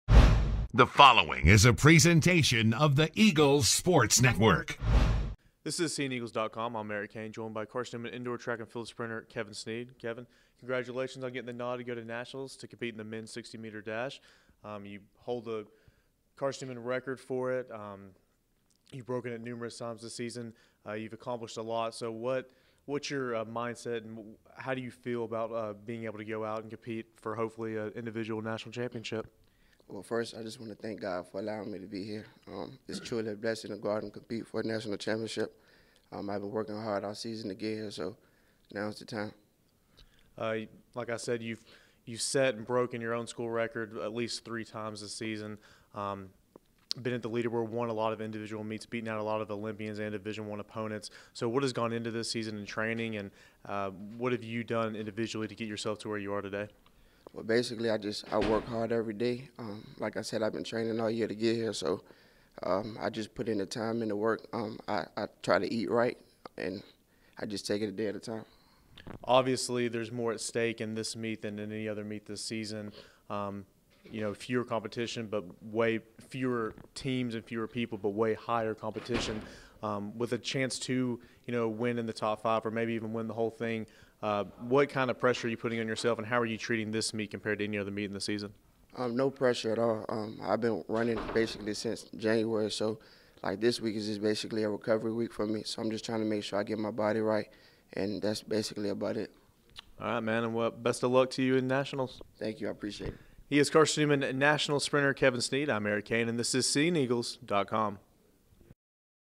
Carson Newman Track and field: Kevin Snead Interview 3-2-16
Kevin Snead chats things up with Eric Cain about competing in nationals on march 11-12.